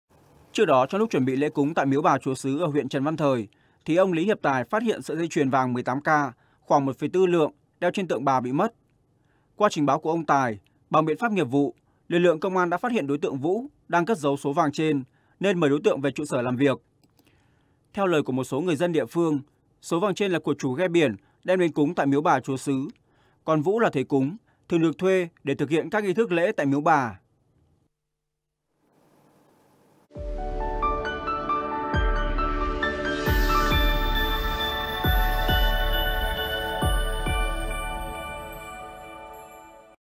Cà Mau: Thầy cúng trộm dây chuyền 1,4 lượng trên cổ tượng miếu Bà Chúa Xứ | VOV
Trước đó, trong lúc chuẩn bị lễ cúng tại Miếu Bà Chúa Xứ ở huyện Trần Văn Thời thì ông Lý Hiệp Tài  phát hiện sợi dây chuyền vàng 18K khoảng 1,4 lượng đeo trên tượng Bà bị mất. Qua trình báo của ông Tài, bằng biện pháp nghiệp vụ lực lượng công an đã phát hiện đối tượng Vũ đang cất giấu số vàng trên nên mời đối tượng về trụ sở làm việc.